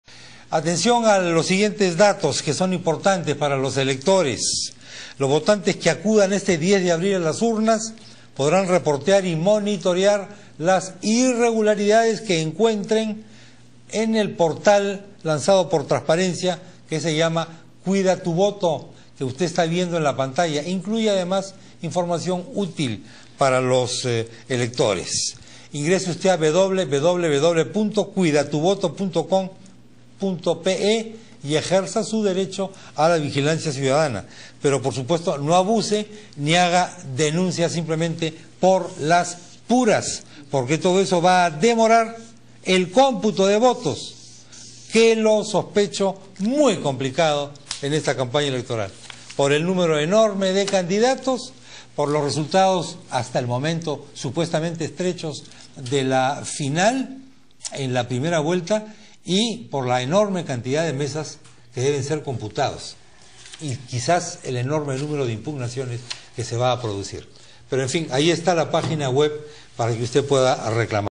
Transparencia lanza portal web Cuida tu Voto en RPP TV
Los peruanos y peruanas que acudan este 10 de abril a las urnas podrán reportar y monitorear las irregularidades electorales en un novedoso portal web llamado Cuida Tu Voto impulsado por la Asociación Civil Transparencia con el fin de promover la vigilancia ciudadana. La iniciativa busca que cada persona que se presente a votar se convierta en un observador ciudadano, pues de lo que se trata es de hacer un retrato del proceso electoral desde el punto de vista del elector.